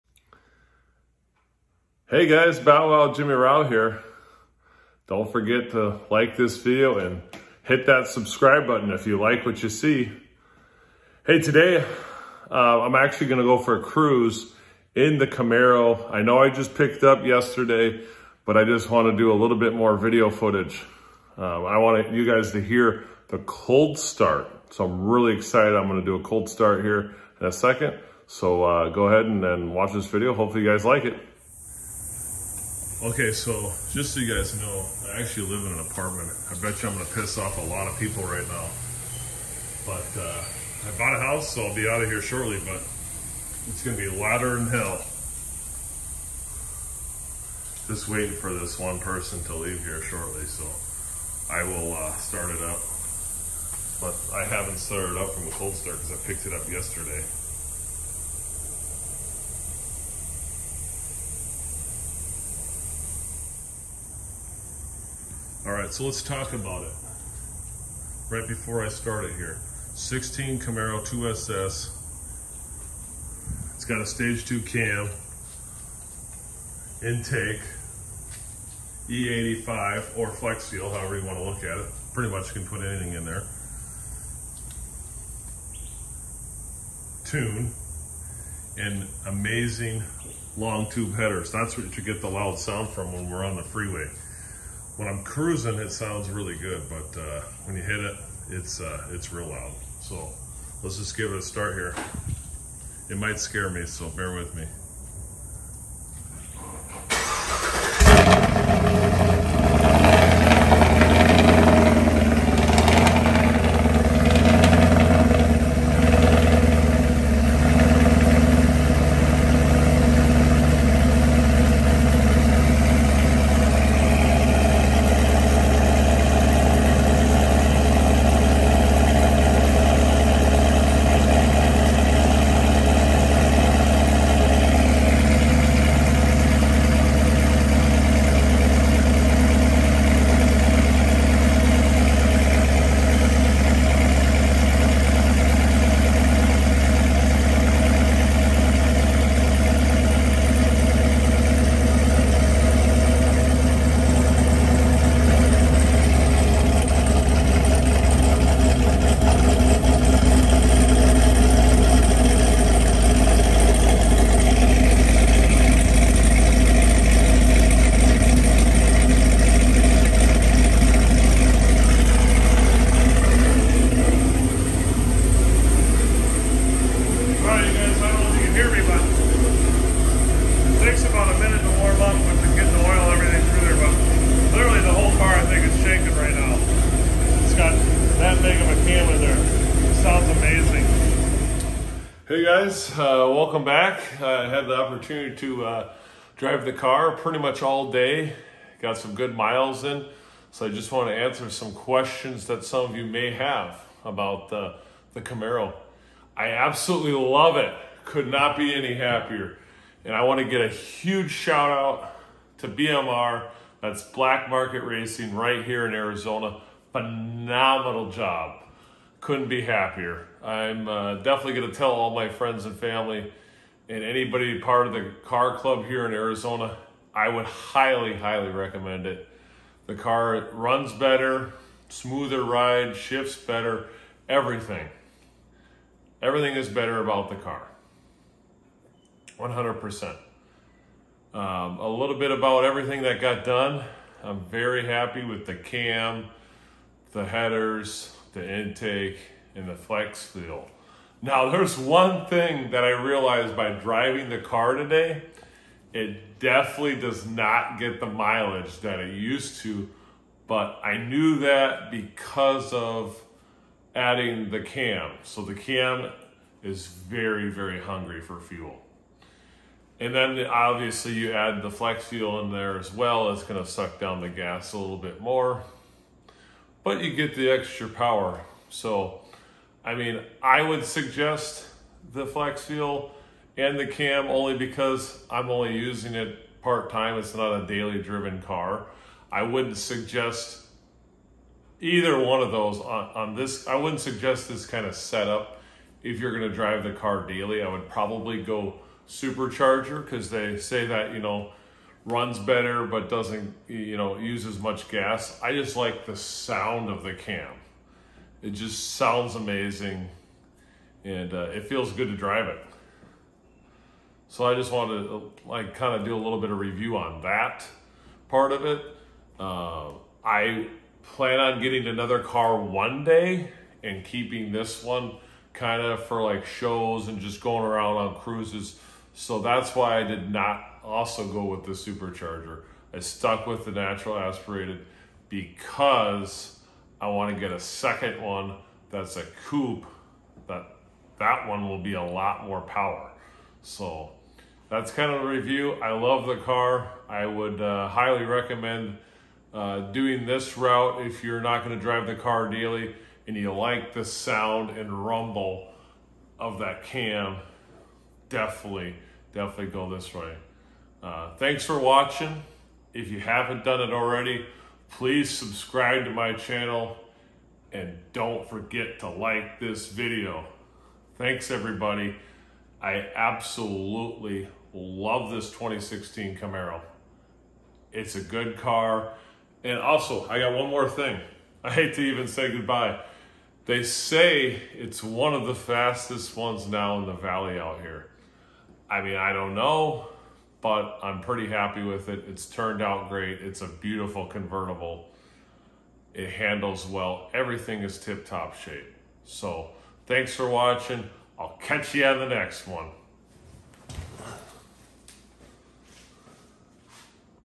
stage 2 cam# 16 Camaro Convertible#
Took car for a drive today. Runs even better now. Thanks to BMR for doing a great job!
Cam
Headers
Intake
Flex fuel